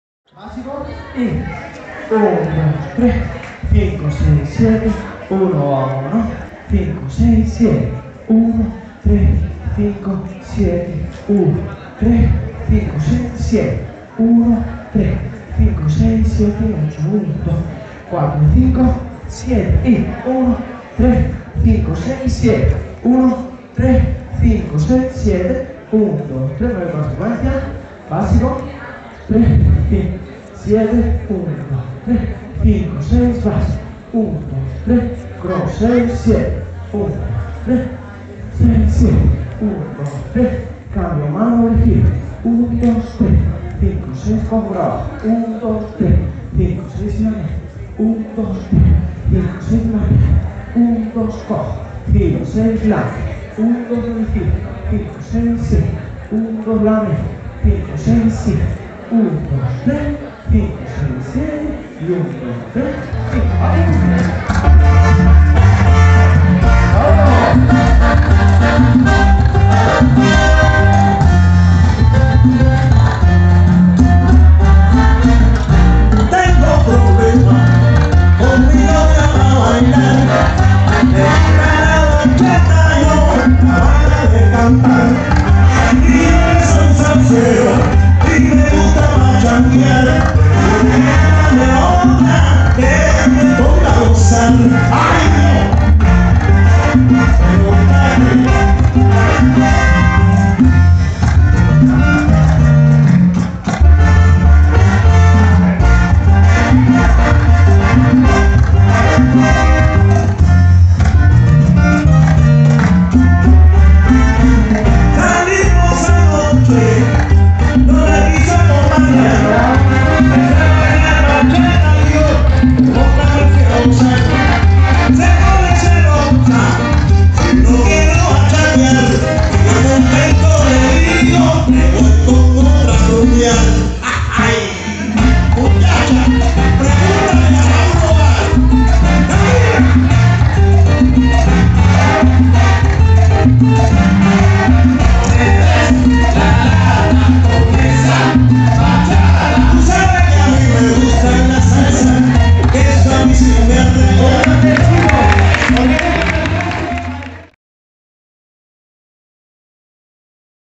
Salsalamanca 2014 Taller David y Maria
Estupendo taller de David y María. Campeones de España del Salsa Open. 22 de marzo 2014.